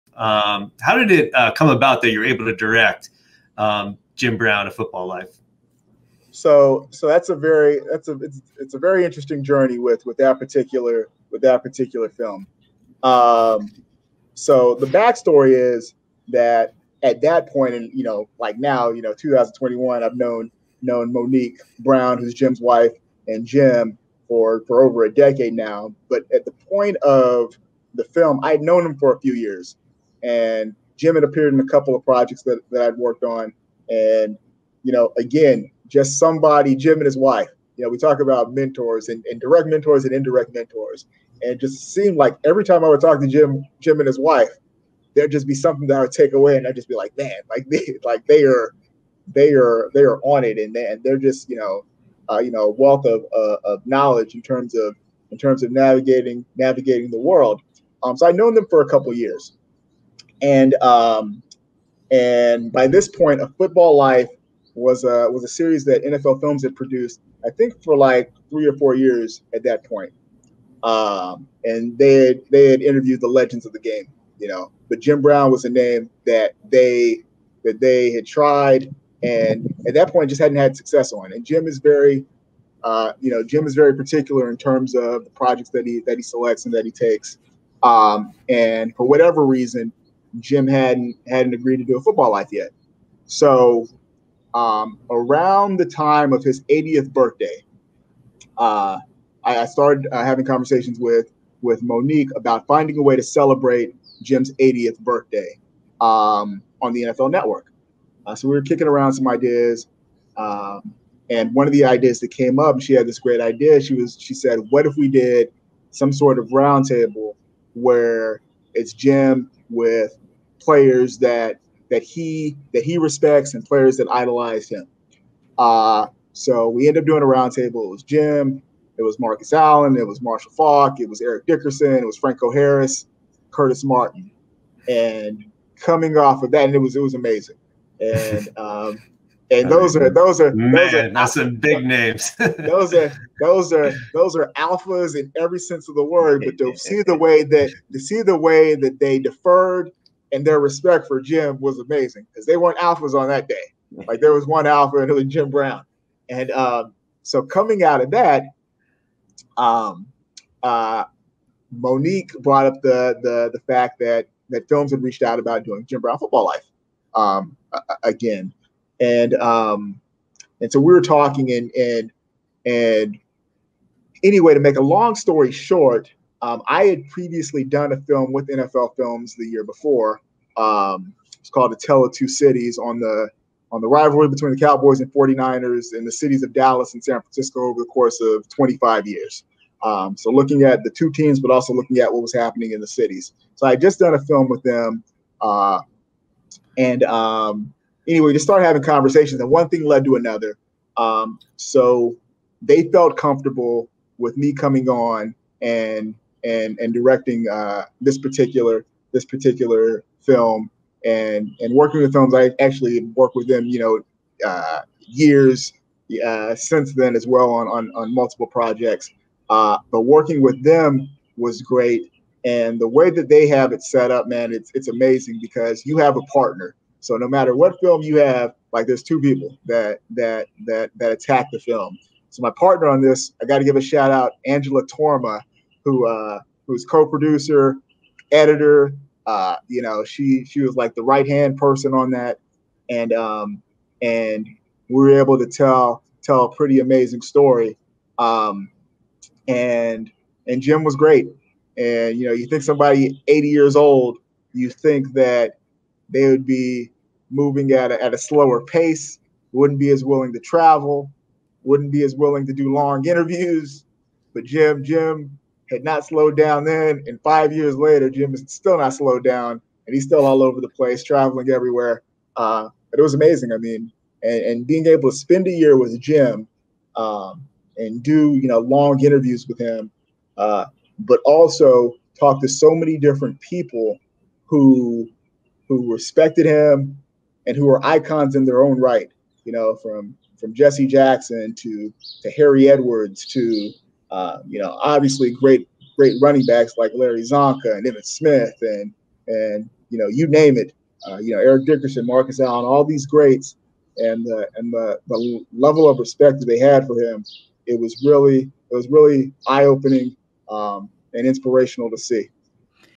Anthony Smith On Getting Involved With "Jim Brown A Football Life"
Award-winning director Anthony Smith, a driving force at NFL Media for the last 16 years, reflects on how he got involved with his film "Jim Brown: A Football Life."

Among his important work in the Social Justice space for the NFL, Smith has directed documentaries about Fritz Pollard, Nipsey Hussle the dynamic Hall of Fame Running Back, Jim Brown, as part of the NFL Films documentary series A Football Life. Smith has been nominated for 7 Sports Emmys Awards.